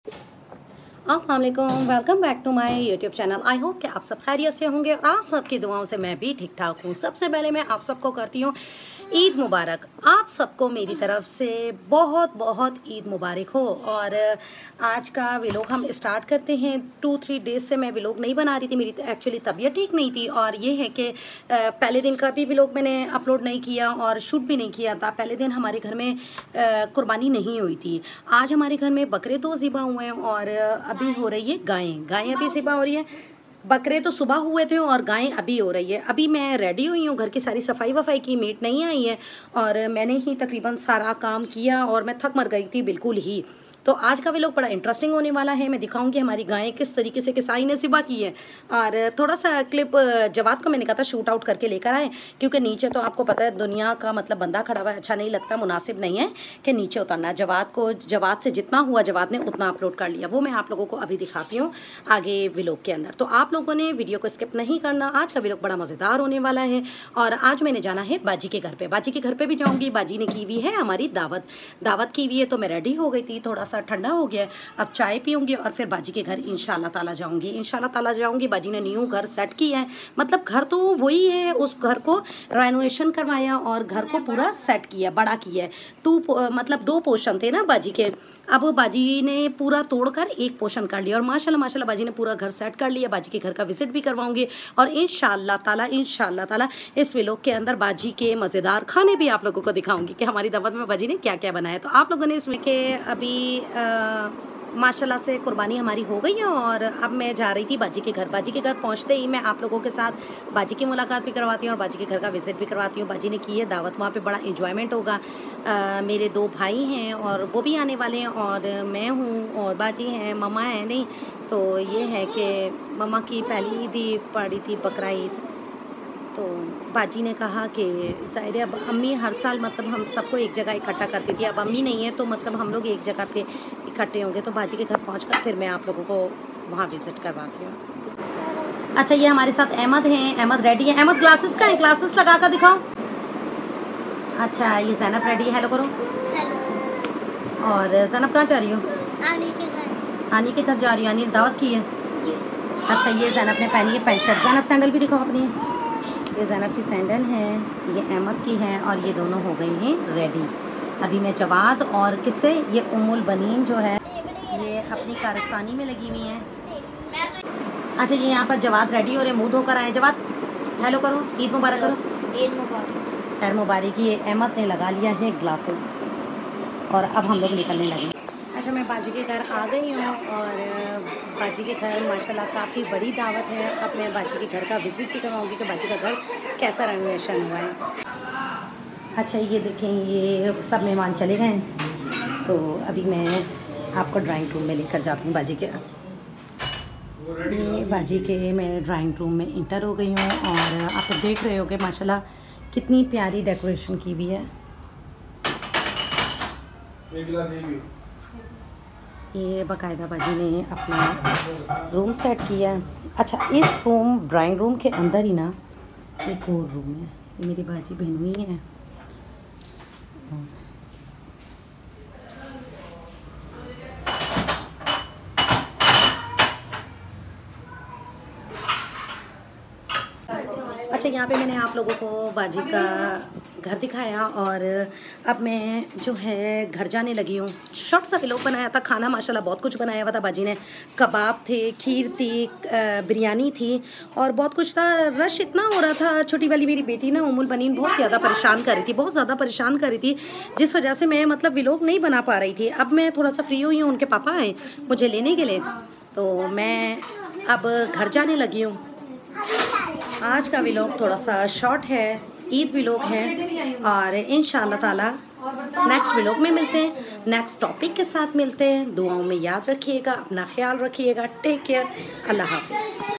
Eid 2nd Day Vlog With Mahnoor | Enjoyment with Sister | Bhot Maza Aya
Aj Eid ka 2nd day ha Or hmaray ghar main qurbani hoi bji ny dawat py bhi bolyia tha bji ka new ghr set howa jis ki waja sy aunhon ny dawat ki tha j bji ka home tour bhi krwa rhe hon ghar ka